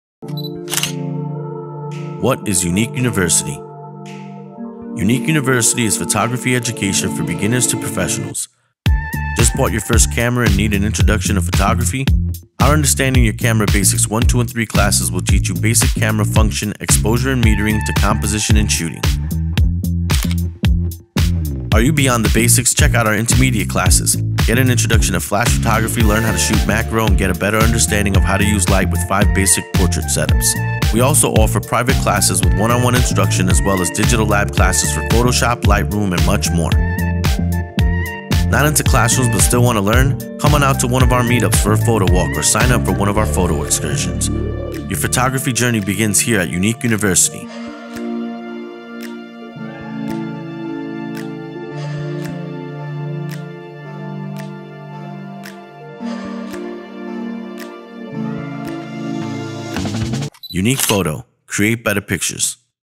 What is Unique University?
Unique University is Photography Education for Beginners to Professionals.

If you just bought your first camera and need an introduction to photography,
our Understanding Your Camera Basics 1,2 & 3 Classes will teach you Basic Camera Function, Exposure & Metering to Composition & Shooting.

Are you beyond the basics? Get an introduction to Flash Photography, Learn how to shoot Macro and get a better understanding of how to use light with 5  Basic Portrait Setups.

We also offer Private Classes with One on One instruction as well as Digital Lab Classes for Photoshop, Lightroom and much more.

Not into classrooms but still want to learn? Come on out to one of our Meetups for a Photowalk  or Sign up for one of our Photo Excursions.

Your photography journey begins here at Unique University.

Unique Photo

Create Better Pictures!